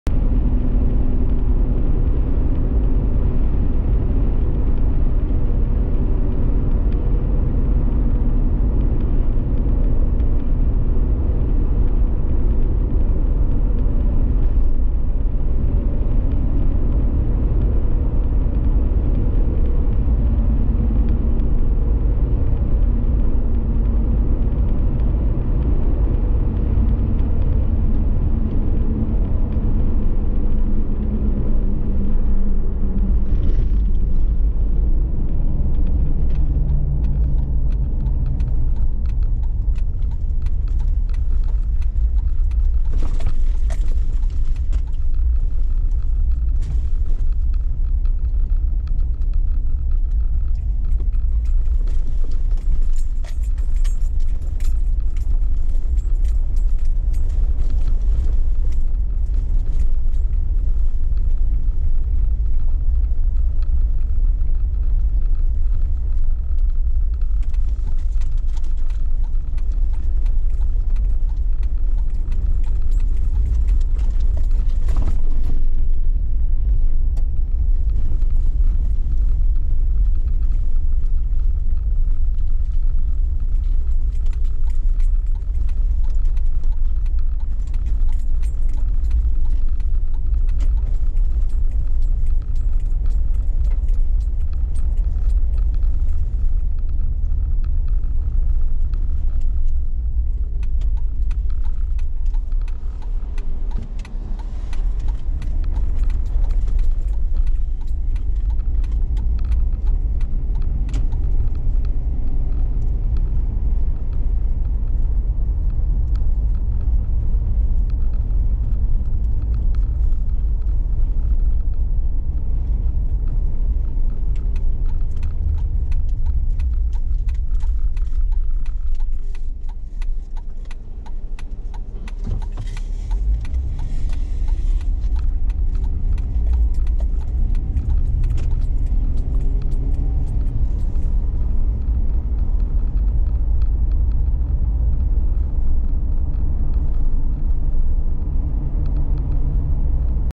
Plummer, ID America's Small Towns
Plummer is a city in Benewah County, Idaho, United States. The population was 1,044 at the 2010 census, up from 990 in 2000